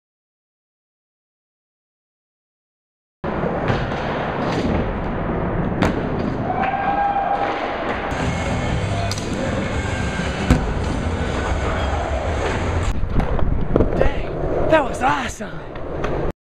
Joey Adamczyk two trick clip
Joey adamczyk two trick clip at the office skatepark.
switch fronside flip followed by a hardflip revert ( ghetto bird.?)